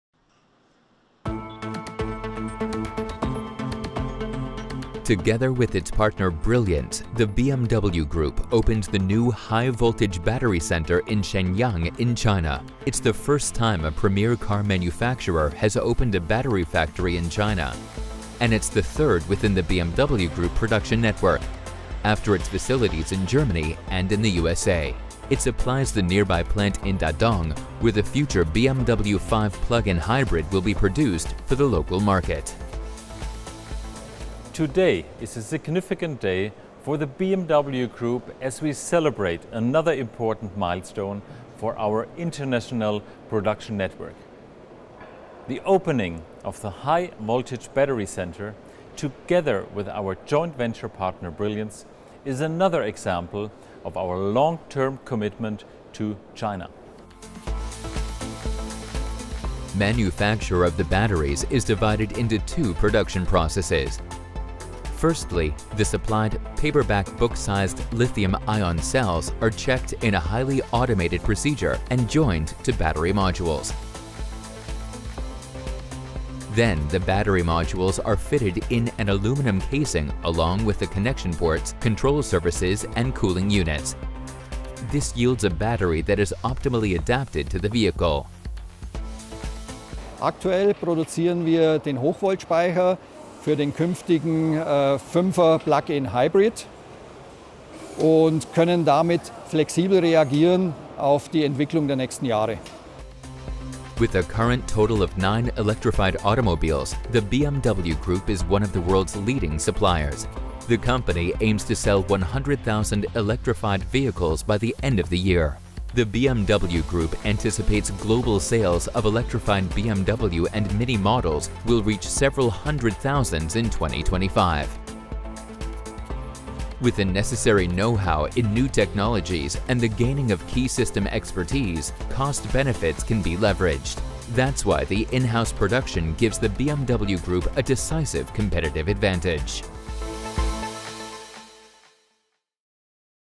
BMW Brilliance Automotive opens battery factory in Shenyang
BMW Group continues to expand its electro-mobility activities. In conjunction with its partner, Brilliance China Automotive Holdings Ltd., the BMW Group opened a new “High-Voltage Battery Centre” in Shenyang, China. The battery factory will supply the nearby Dadong plant, which is operated by the BMW Brilliance Automotive (BBA) joint venture and will produce the BMW 5 Series Plug-in Hybrid for the local market.